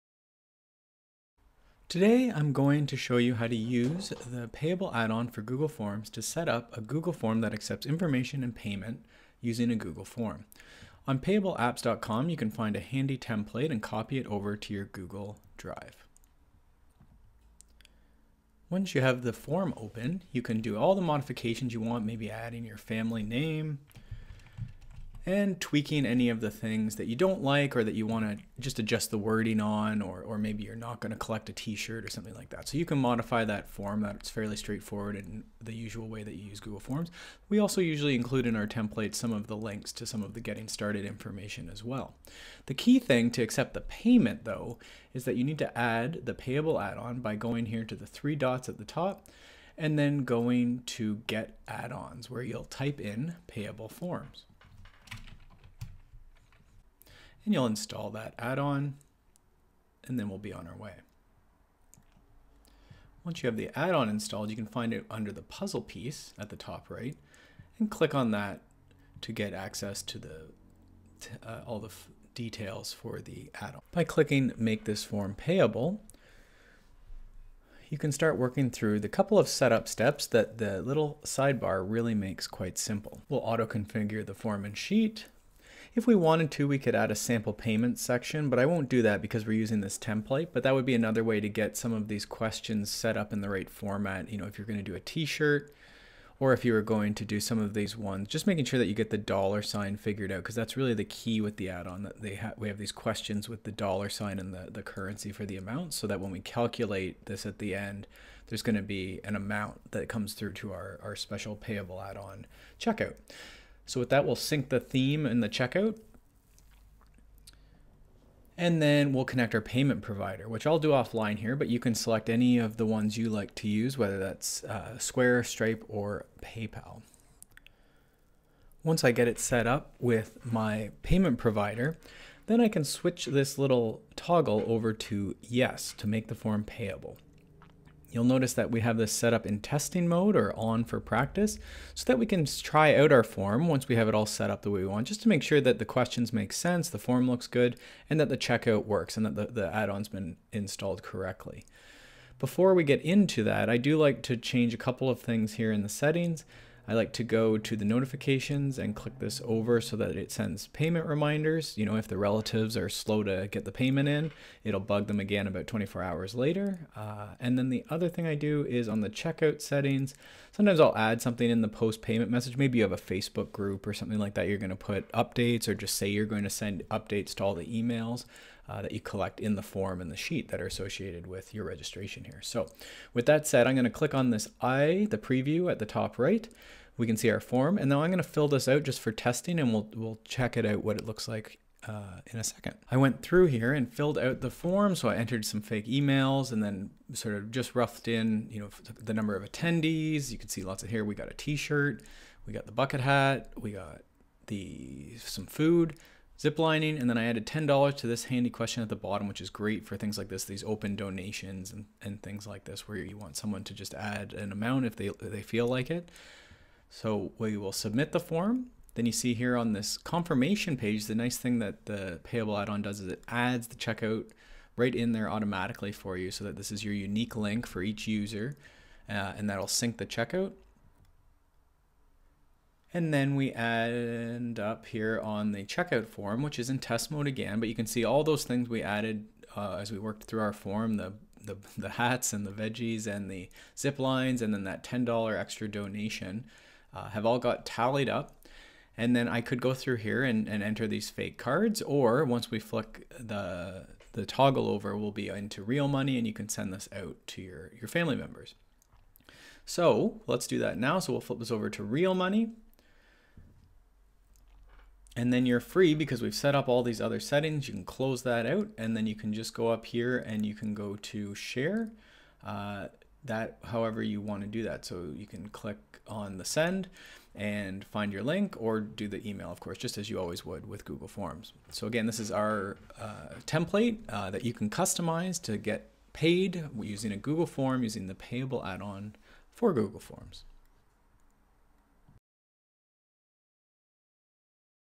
Organize Your Family Reunion in Google Forms
How to use Google Forms and the Payable Add-On to organize your family reunion—and make sure everyone pays their share!